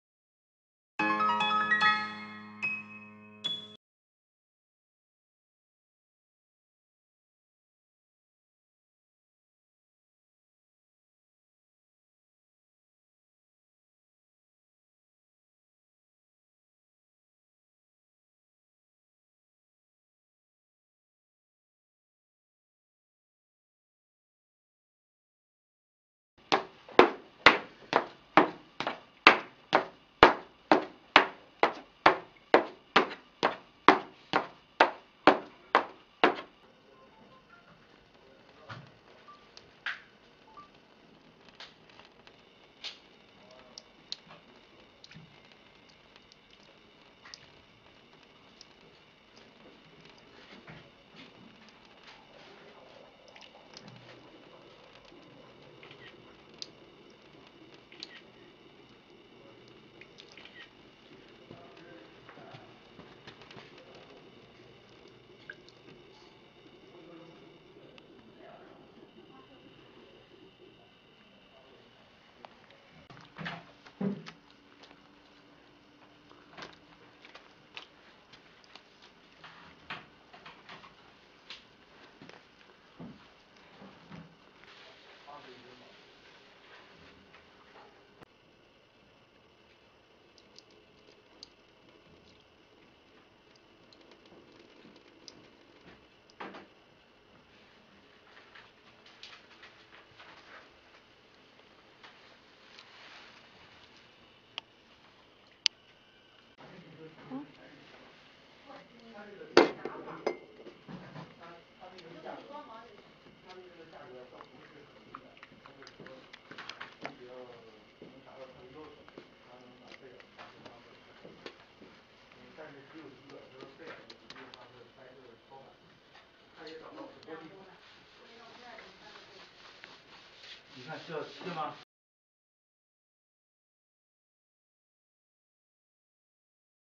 Waterproof Laminate Flooring---DECNO GROUP
New Generation Flooring.
DECNO Waterproof Laminate flooring, which is a new generation of floor. Black HDF boards, with the Atomic hydrogen and high density, is resistant to moisture.
With their waterproof construction and leading hardwood designs, it is an  ideal flooring options for any room - even bathrooms, kitchens and mudrooms.
When it's waterproof, it's worry-proof!
1). Super water resistance: ≤3.9% of thinkness swelling rate after 24 hours in water;
2). Super high density: ≥ 1100KG/m3 HDF;
3). Formaldehyde emissions: E0 standard;
4). Widely Use: Can be installed in the kitchen and bathroom;
5). Instead Marble: Can be produced Marble designs;
6). Anti-mildew;

Any requirements, please contact with me directly.